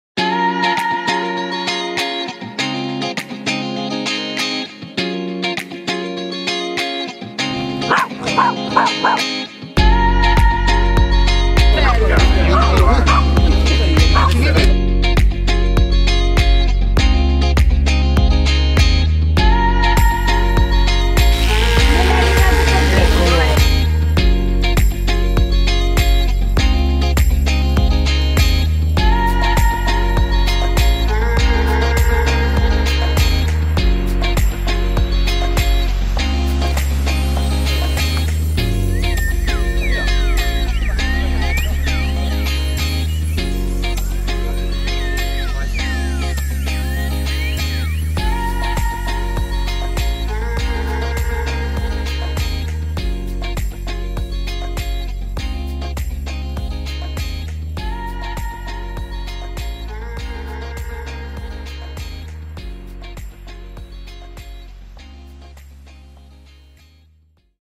움브리아 트러플 파인더 협회의 대회 - 훈련된 똑똑한 강아지들 키운다 - 송로버섯
지난 5월에 움브리아에서 트러플 파인더 대회가 열렸습니다. Selezione Umbria 직접 가서 실제로 보고 촬영하고  왔습니다. 트러플에 대한 더 많이 알아보고 새로운 것을 알게 되었습니다.